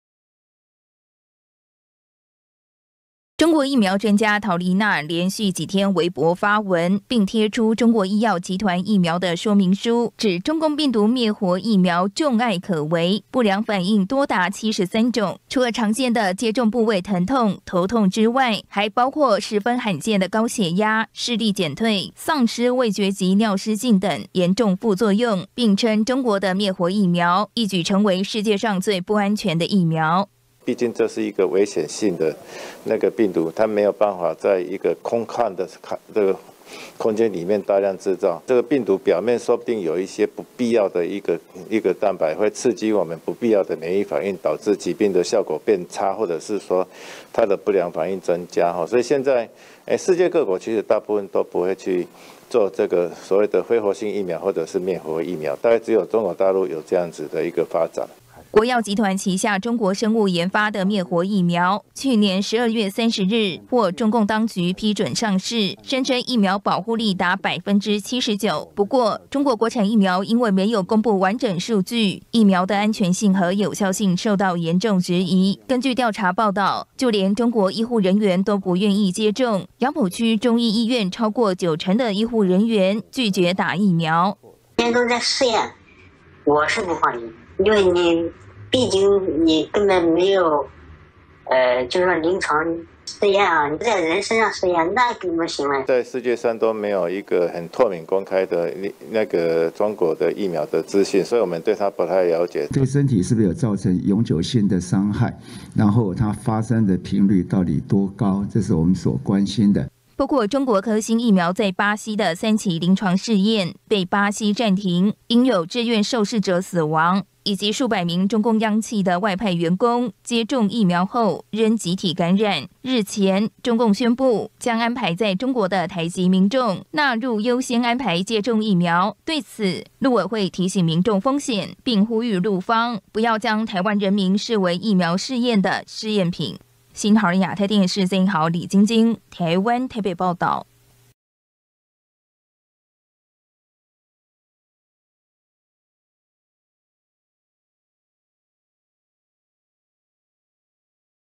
質疑聲不斷！中國疫苗73種副作用 世上最不安全｜@ChinaBreakingNewsTV ｜20210107
中國疫苗，面臨質疑聲不斷。中國醫藥集團(簡稱：國藥集團)研發的中共病毒滅活疫苗，上週三(12月30日)獲得中共當局批准上市。不過，昨天(5日)中國疫苗專家陶黎納，在網上貼出國藥集團疫苗的說明書，內容提到，接種後的副作用多達73種，是世界上最不安全的疫苗。PO文引起中國網民熱烈討論，但5日晚間已經全被當局刪除。